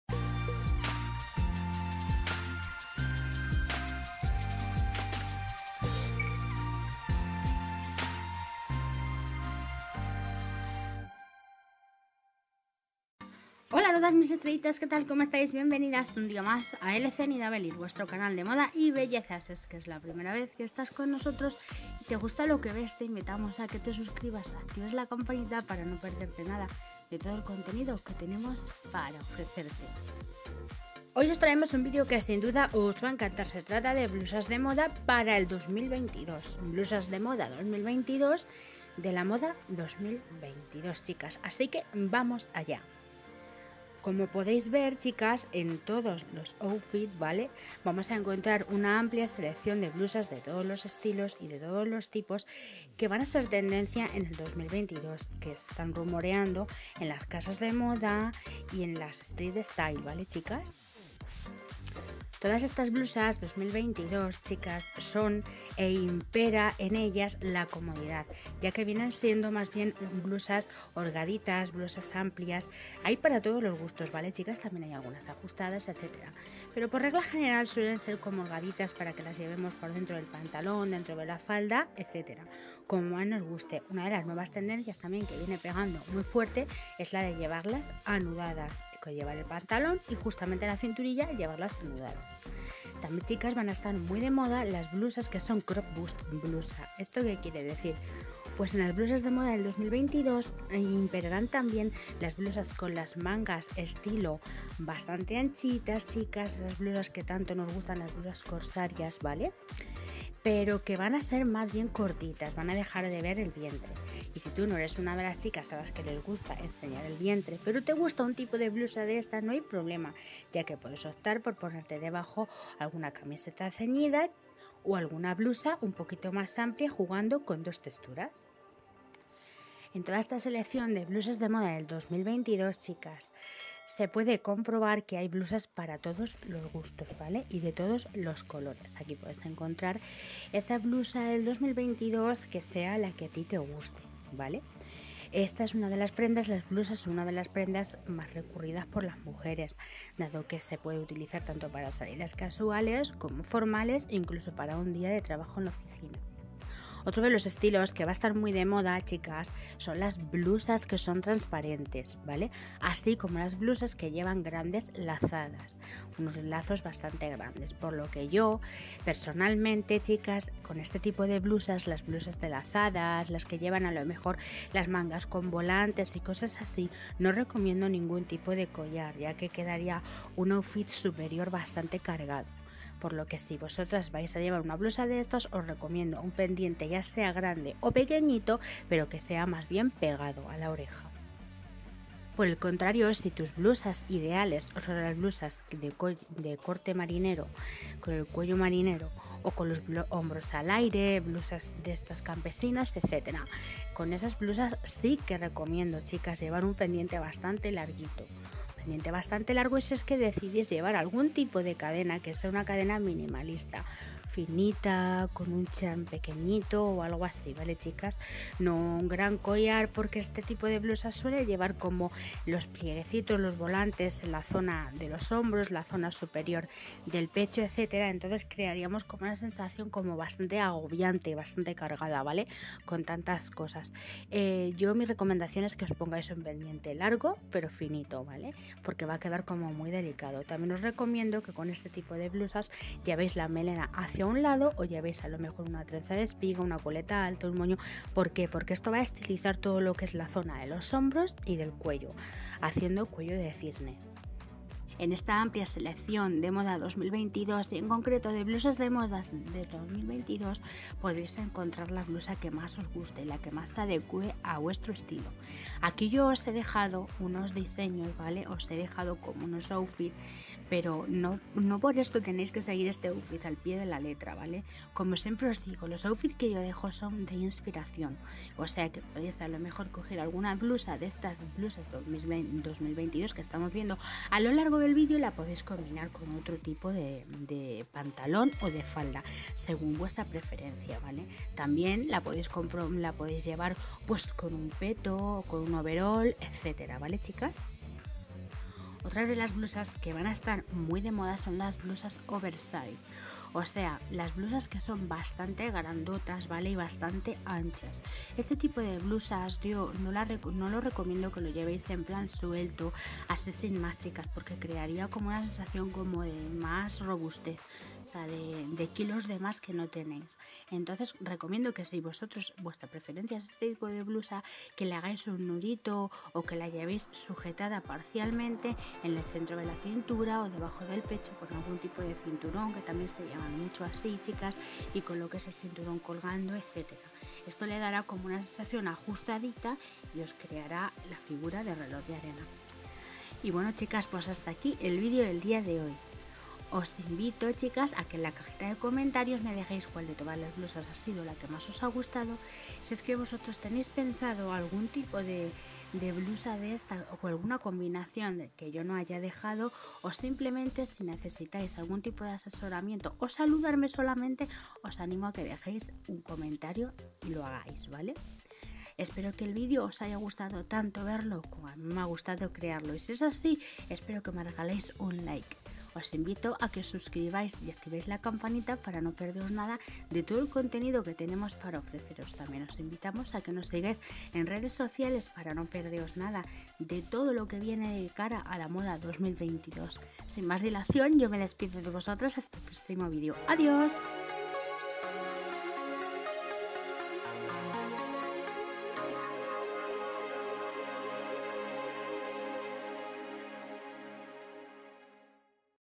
BLUSAS DE MODA 2022 - MODA 2022 - BLUSAS 2022 - MODA 2022 MUJER
En esta ocasión os traemos una primicia total en blusas de moda 2022, todas ellas un referente en la moda 2022. Estas blusas 2022 son de todo tipo, estilos y colores por lo que toda mujer indiferentemente de la edad y físico que tenga podrá encontrar aquí sus blusas de moda 2022 ideales para lograr su estilo y conseguir un look elegante y sofisticado. También podréis inspiraros para crear vuestros propios outfits de moda 2022. Os dejaremos blusas de todo tipo y para todas las estaciones de año y temporadas.
Si deseas estar a la ultima  moda 2022 y saber como combinarla quédate conmigo y te explico como hacerlo. Besazos!!